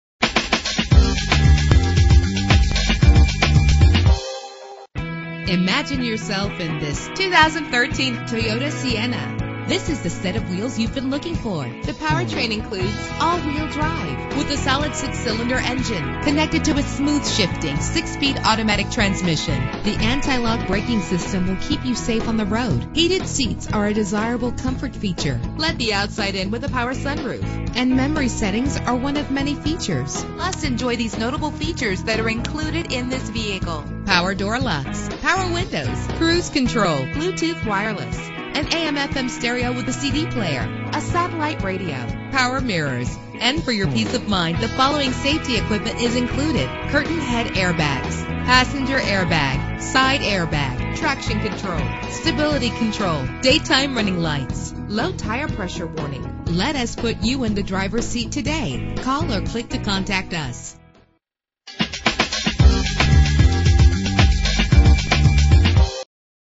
2013 Toyota Sienna - Avenel New Jersey
Year: 2013
Make: Toyota
Model: Sienna
Trim: XLE 7 Passenger
Engine: 3.5 liter 6 cylinder 24 valve
Transmission: 6-Speed Automatic
Color: Super White
Mileage: 0
Address:
90-100 Route 1 North